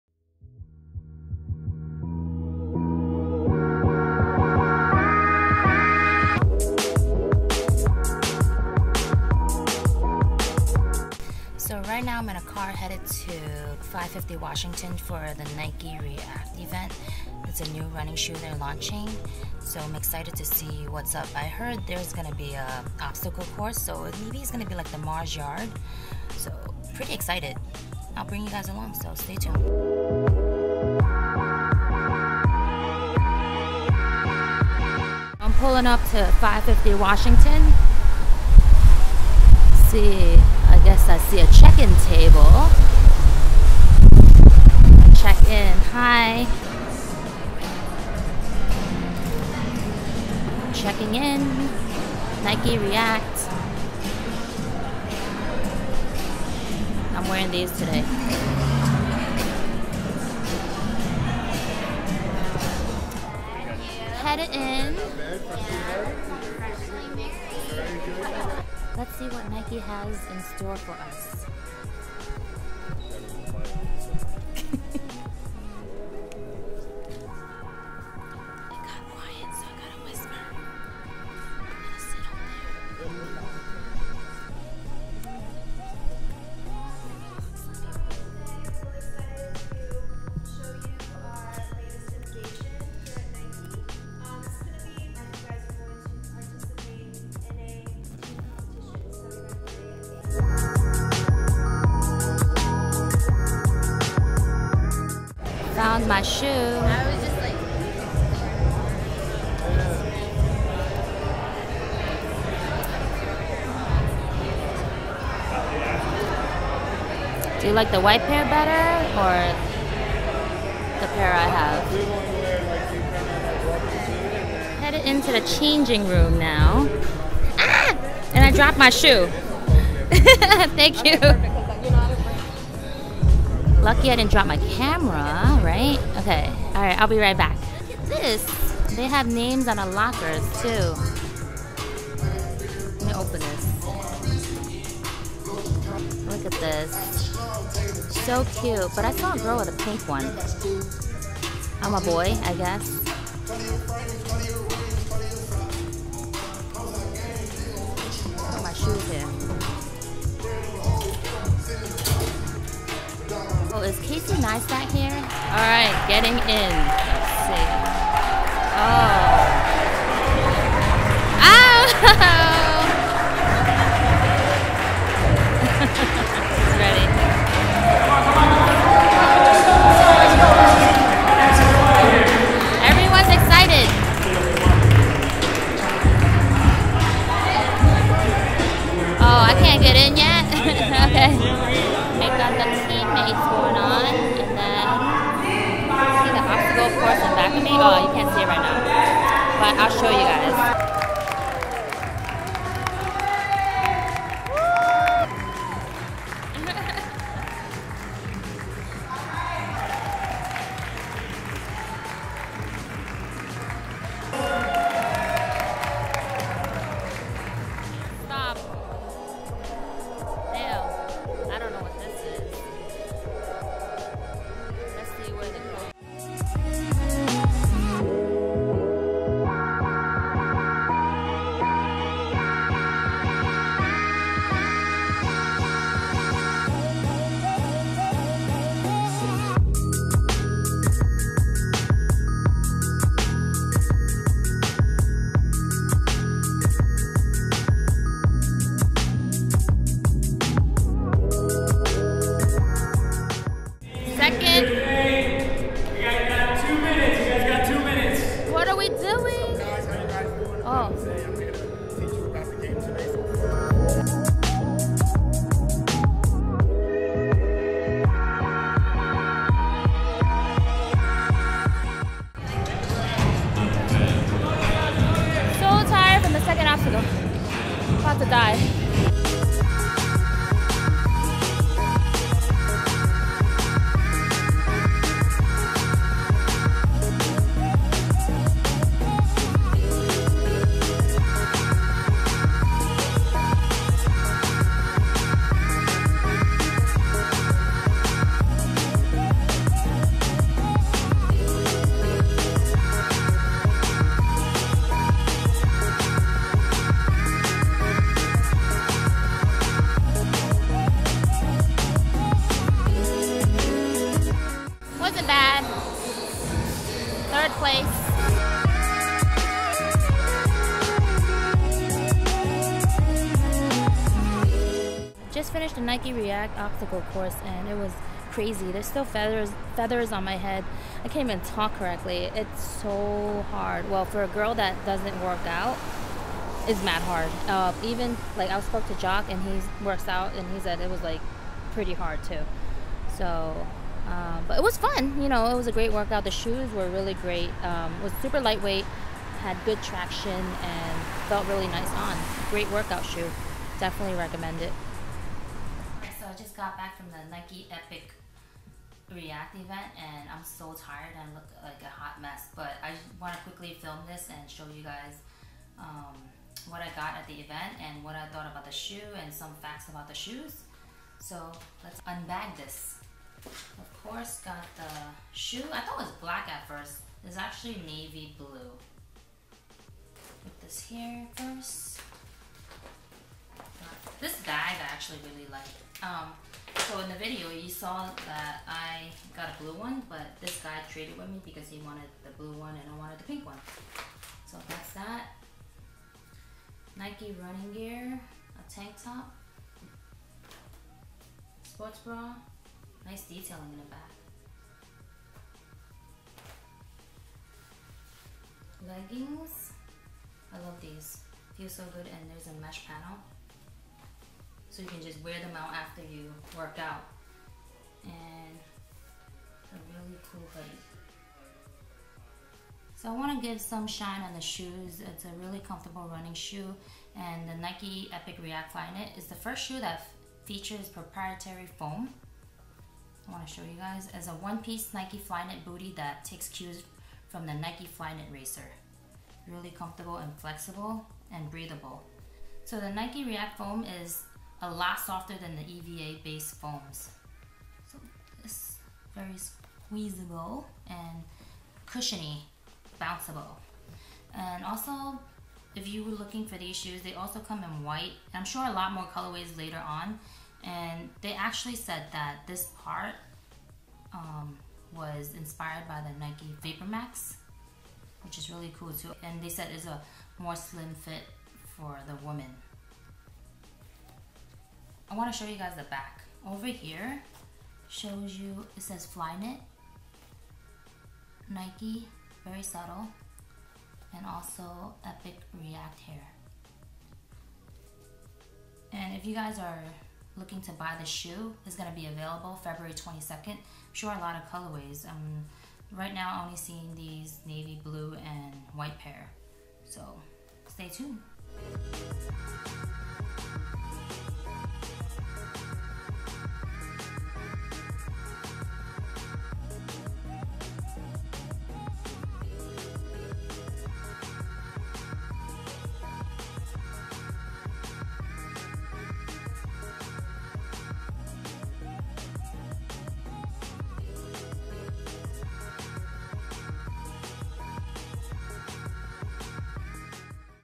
Wear Testing Nike REACT. LOVE IT! | nitro:licious
Crazy wear-test for the NIKE EPIC REACT FLYKNIT today!